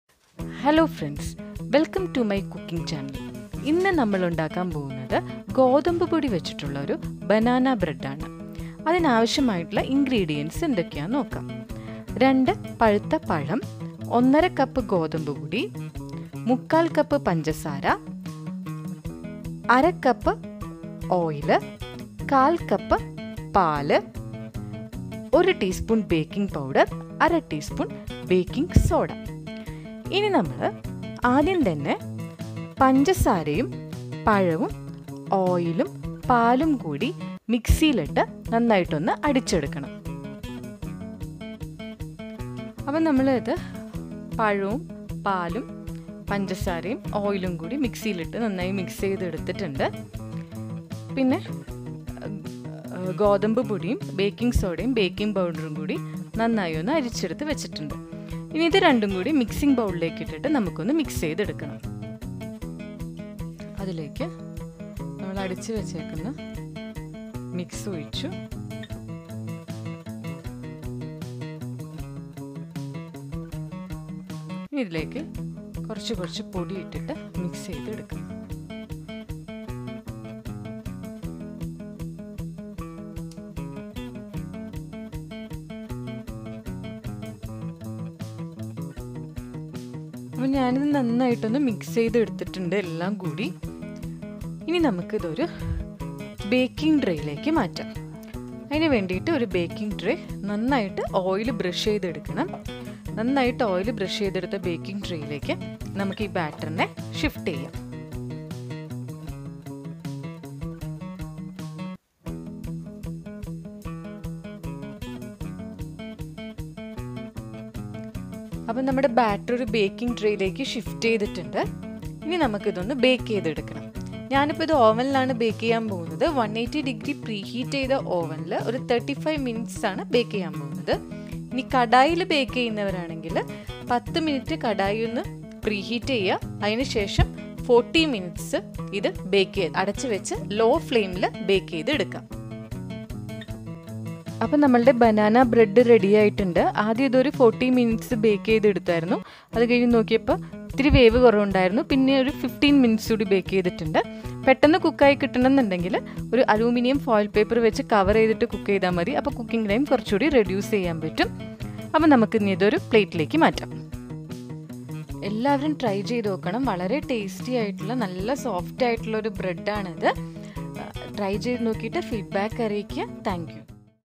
Eggless Wheat Banana Bread | ഗോതമ്പ് പൊടി കൊണ്ടൊരു ബനാന ബ്രഡ്
Ingredients
 Banana – 2 nos
 Wheat Flour – 1.5 cups

About Us: At Swetha's Daily Kitchen, you will find delicious food recipes that are easy to make and prepared with easily available ingredients. Happy Cooking!